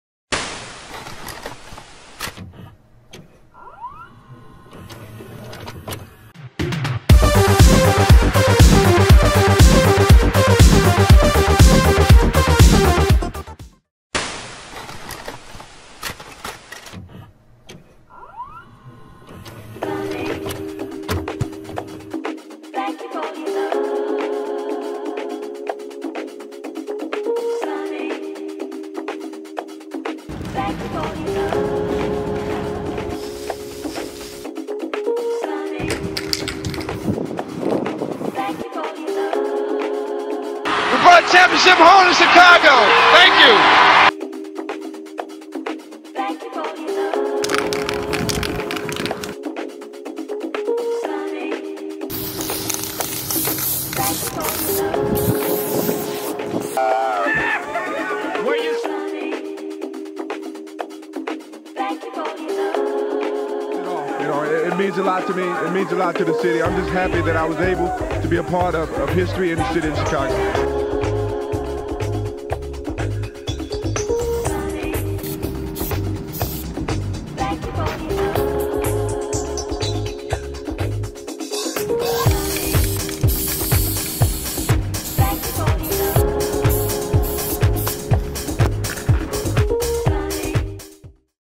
The Air Crew
Upcoming trailer for short film Starring Chicago Crew (AIR CREW)
Filmed & Edited By Bazookafilms77